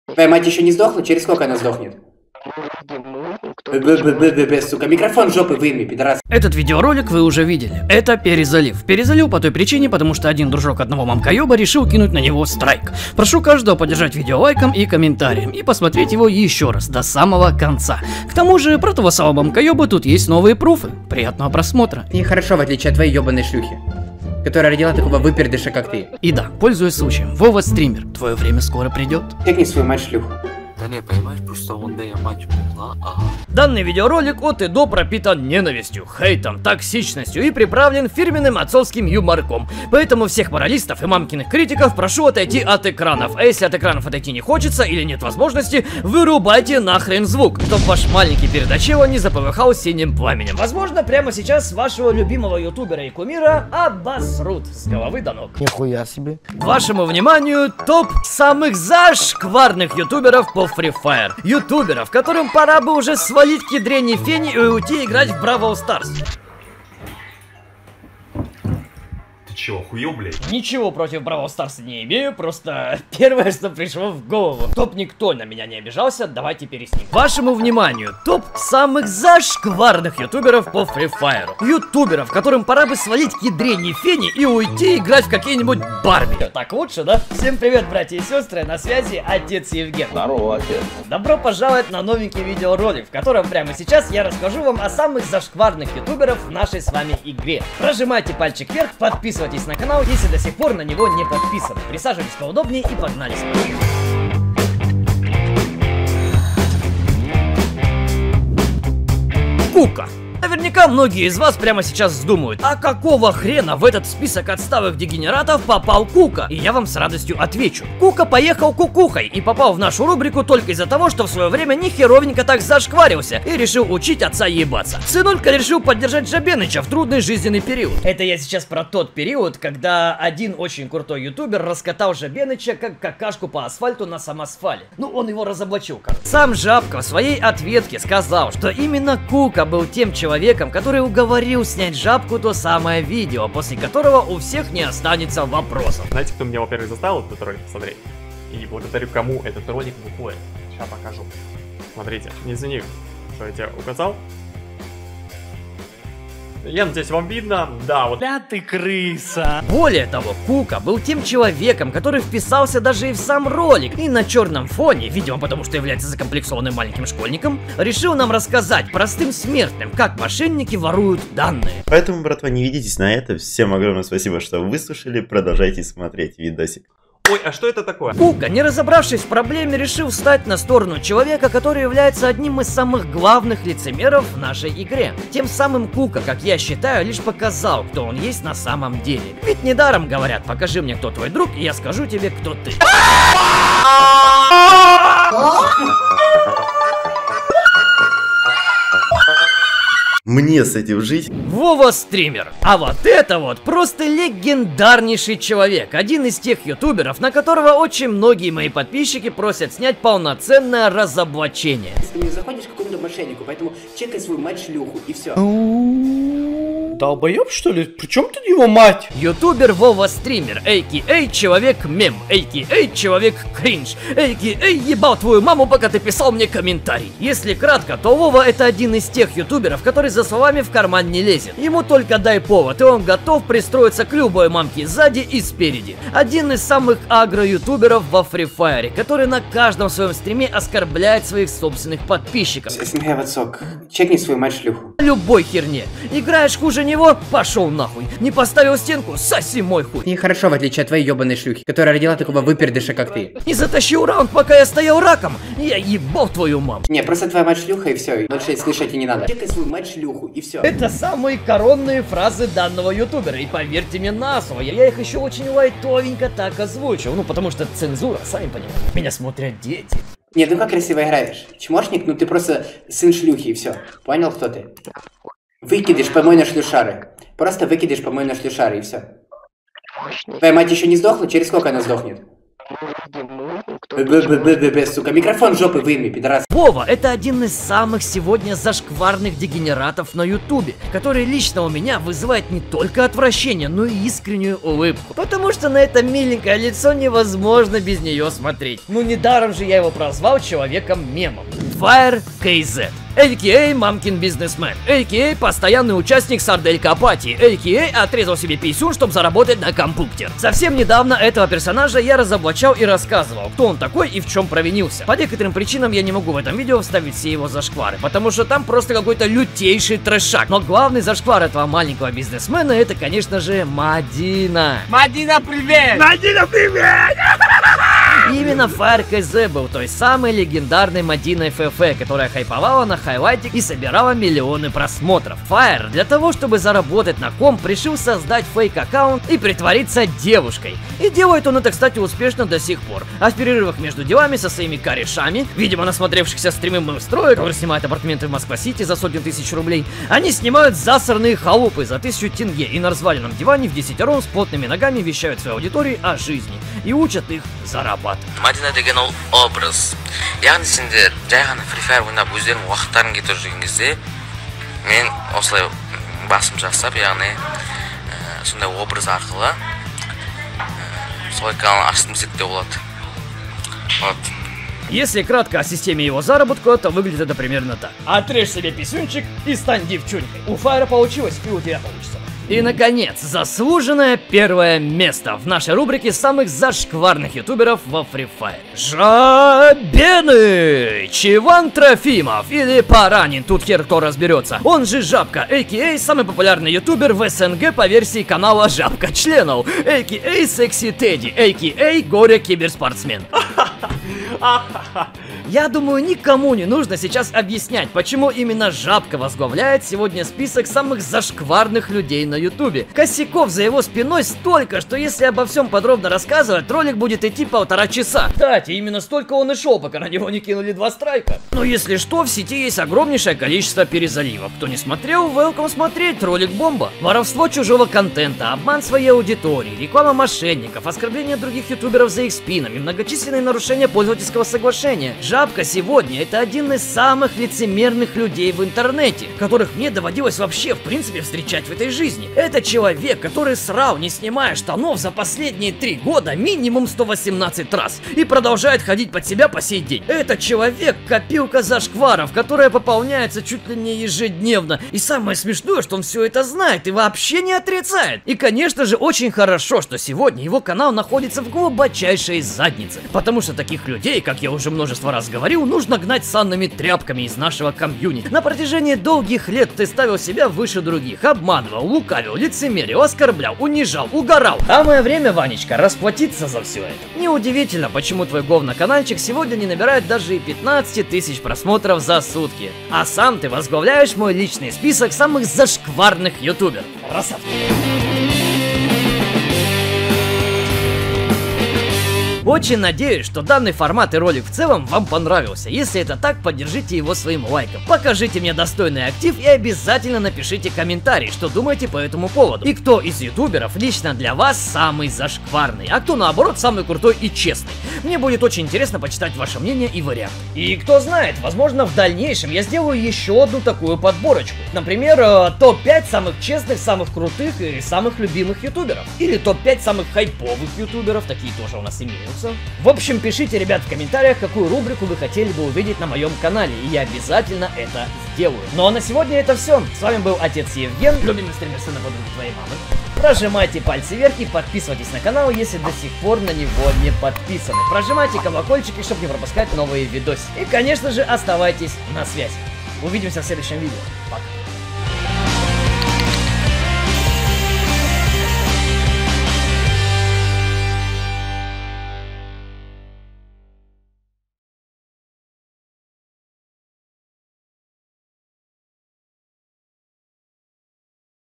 САМЫЕ ЗАШКВАРНЫЕ ЮТУБЕРЫ ФРИ ФАЕР / FREE FIRE
Мой личный топ
САМЫЕ ЗАШКВАРНЫЕ ЮТУБЕРЫ ФРИ ФАЕР
Вова Стример, скоро на тебя полноценное разоблачение)))

Видео пришлось перезалить из-за Вовки, который решил на него кинуть страйк.